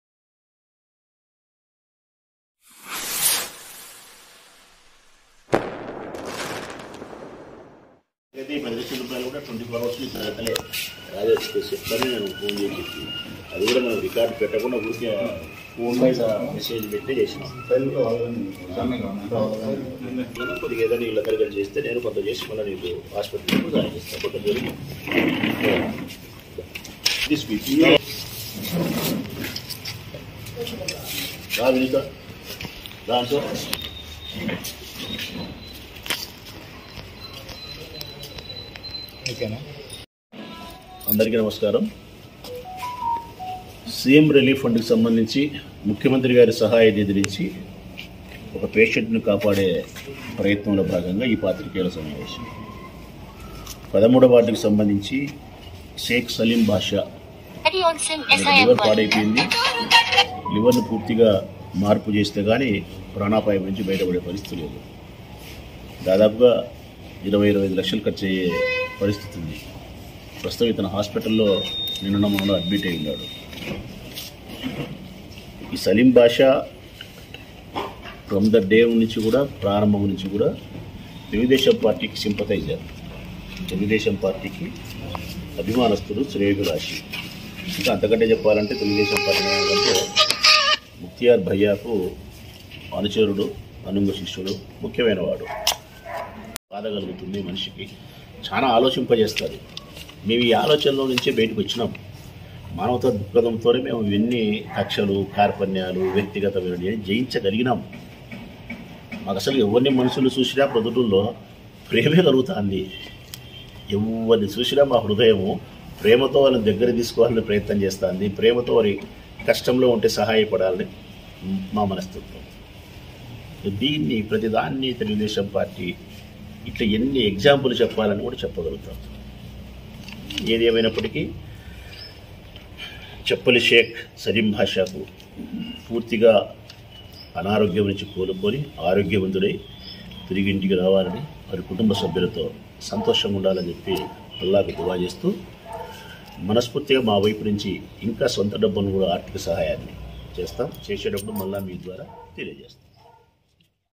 చెప్పలి షేక్ సలీం లివర్ ఆపరేషన్ కోసం 24గం.ల లోపే10 లక్షలు రూ/- లు CMR ఫండ్ చెక్కు పంపిణీ చేసిన.- MLA
చెప్పలి షేక్ సలీం లివర్ ఆపరేషన్ కోసం 24గం.ల లోపే10 లక్షలు రూ/- లు CMR ఫండ్ చెక్కు పంపిణీ చేసిన MLA రాచమల్లు